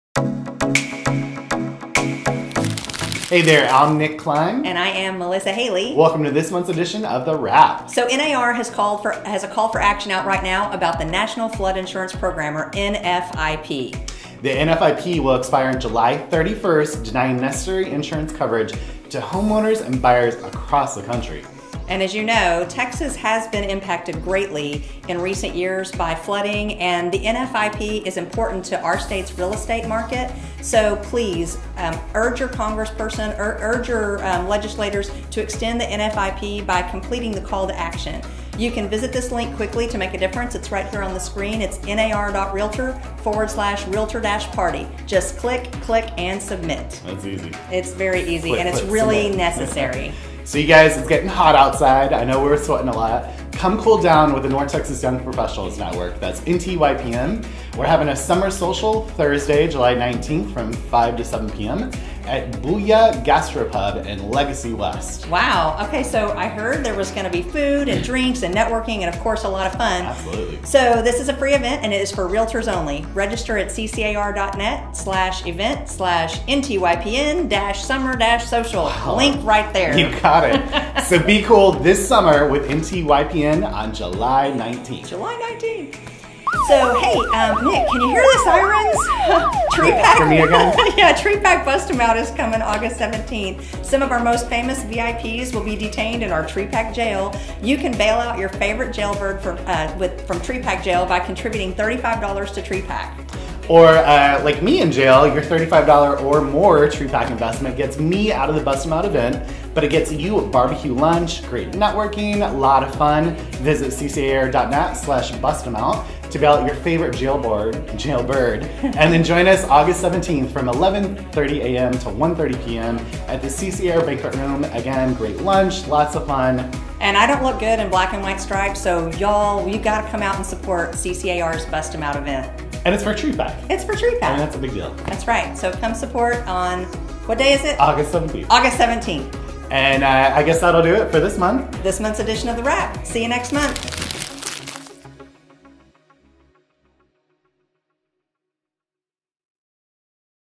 Call for Action and Exciting Summer Events! - The Wrap - S2 E:6
This month on the Wrap we hope you will let your voice be heard with the NAR call for action: National Flood Insurance Program! to urge Congress to extend the NFIP and continue helping those affected by natural disasters.

NTYPN wants you to beat the heat with their upcoming Summer Social on July 19th! 5:00 p.m. to 7:00 p.m. at the Bulla Gastrobar in Legacy West!

TREPAC's annual Bust 'Em Out is almost here! A great and fun way to make your annual contribution to TREPAC and bust out your favorite jailbird! to see a list of all our jailbirds and make your contribution on our before August 17th!
Then join us August 17th in the Banquet room for a barbecue lunch!